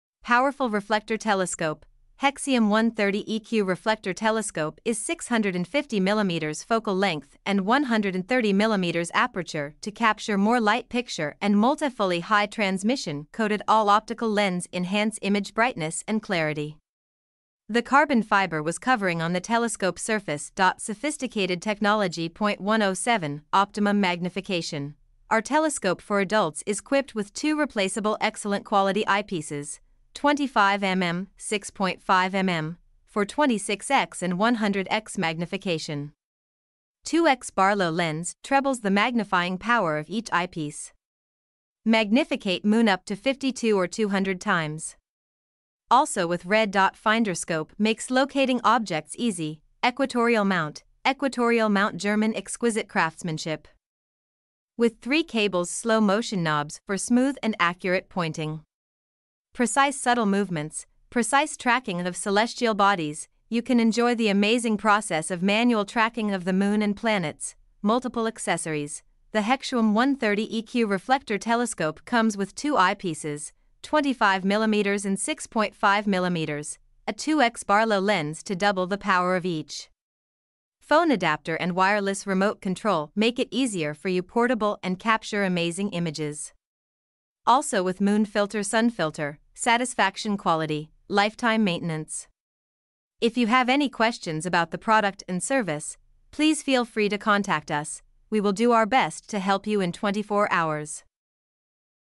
HEXEUM- Telescope 130EQ Astronomical Reflector Telescope
Telescope 130EQ Astronomical Reflector Telescope - Manual Equatorial Telescope for Adults Astronomy. Comes with 2X Barlow Lens Phone Adapter and Moon Filter & Sun Fliter, Wireless Control

*Webliven is an Amazon Associates Program Member.